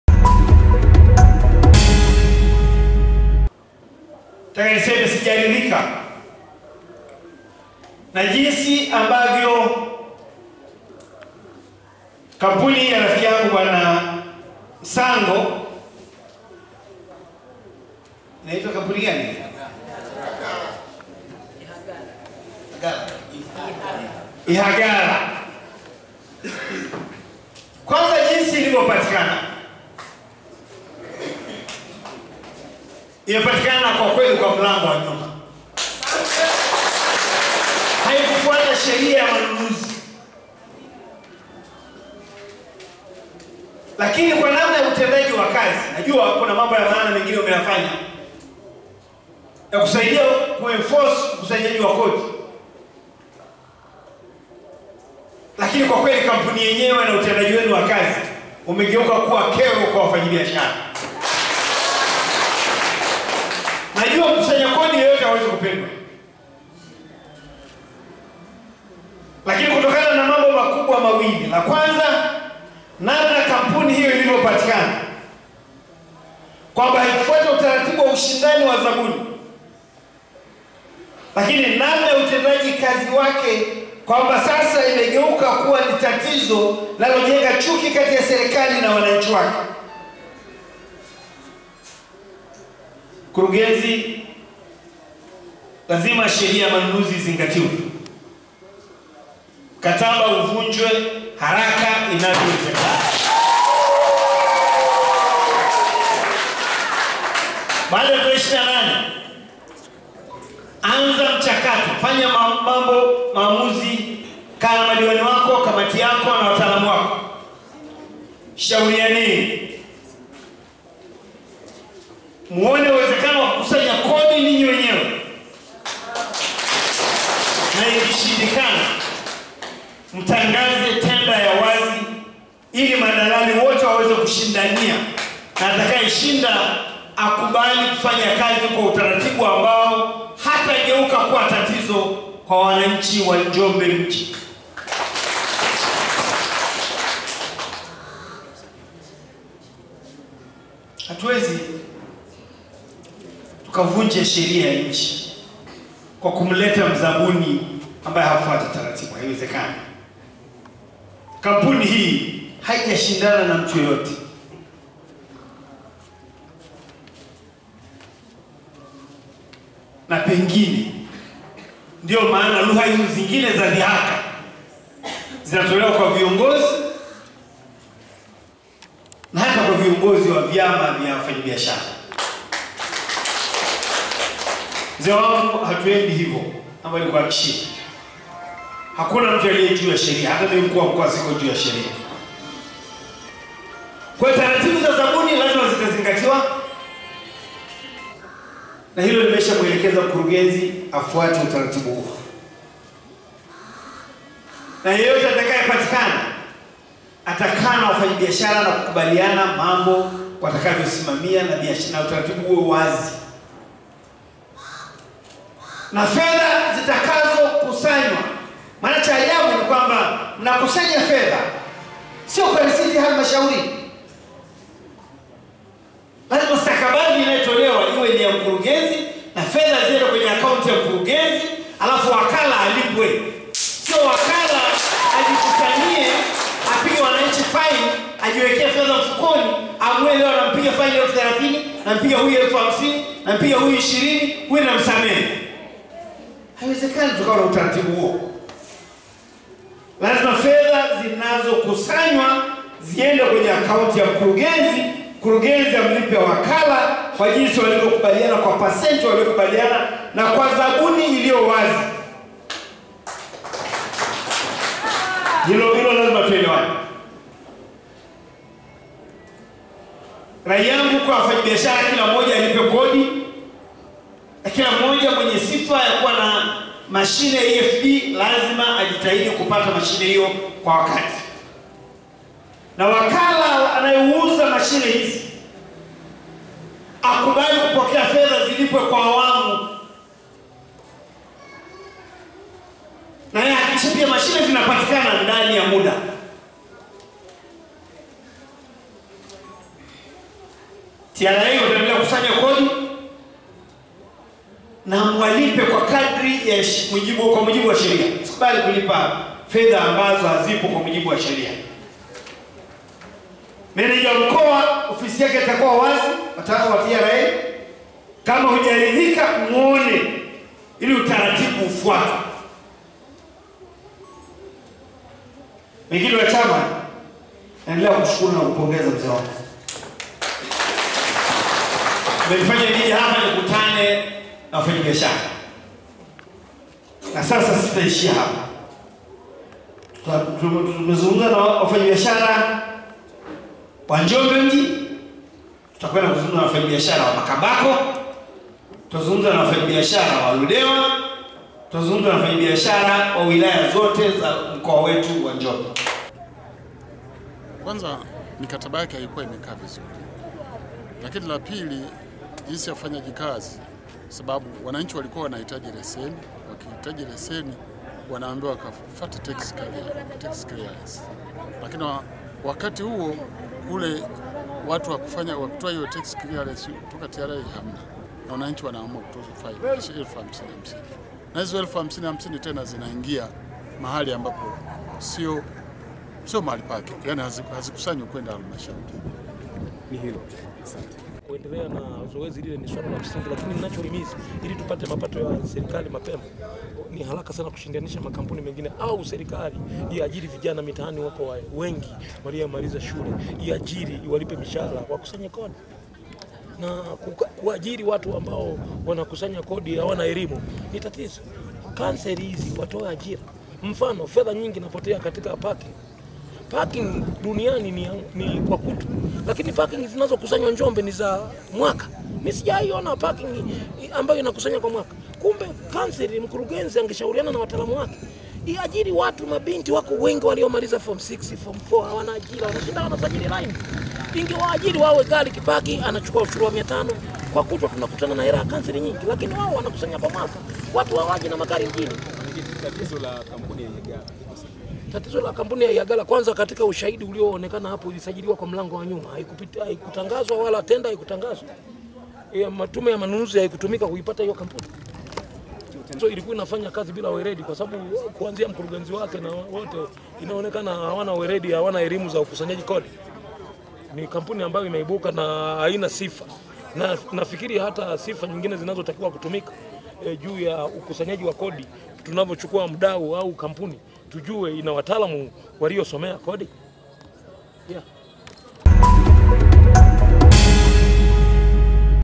MKUU WA MKOA AAGIZA KUVUNJWA MKATABA WA IHAGARA NA HALMASHAURI YA MJI NJOMBE
Elimtaa Tv,
is For News,
Mkuu wa mkoa ameisimamisha kampuni ya Ihagala iliyo kuwa inapigiwa kelele na madiwani katika baraza la madiwani halmashauri ya Mji njombe na kusananisha madiwani watatu kutolewa ambao wanapatikana katika stori hii